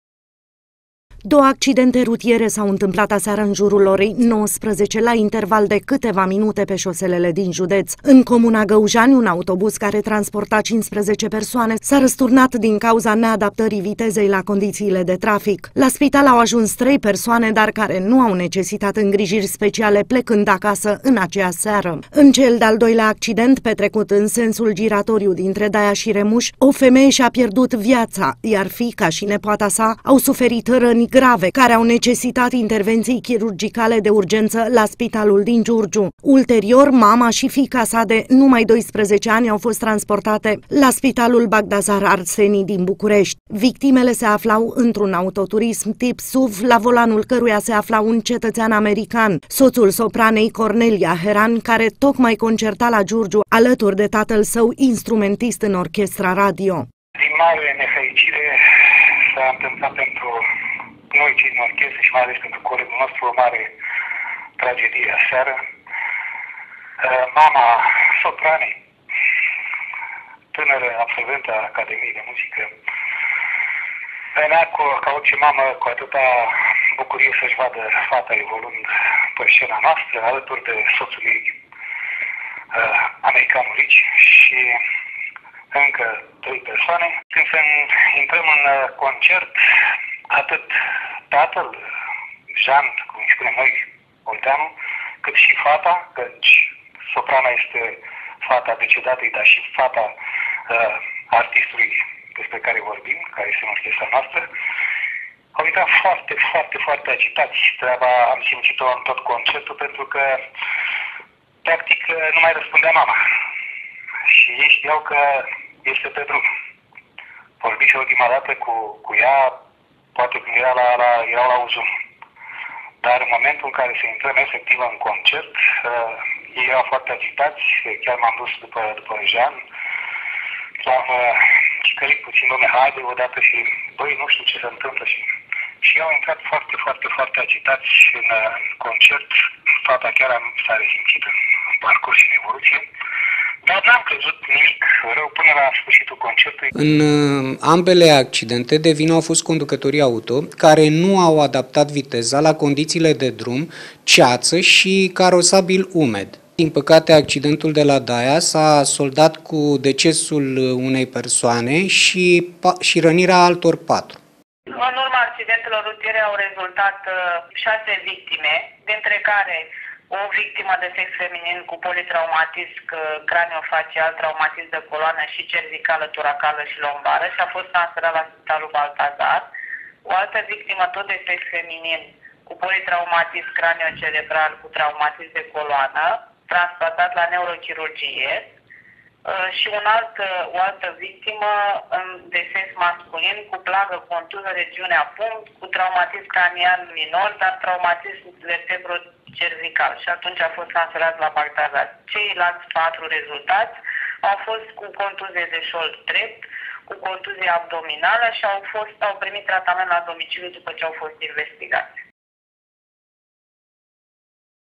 CRONICA NEAGRA PENTRU GIURGIU
În tot orașul au răsunat aseară sirene de ambulanțe. Două accidente rutiere au făcut o cronică neagră, date fiind consecințele. O femeie și-a pierdut viața, în timp ce fiica și nepoata acesteia s-au ales cu răni extrem de grave. Victimele se deplasau spre Giurgiu la concertul de la Ateneul Nicolae Bălănescu. Femeia care a murit, nu a mai apucat să vadă reprezentația fiicei, dar și a soțului său de pe scena Ateneului din Giurgiu.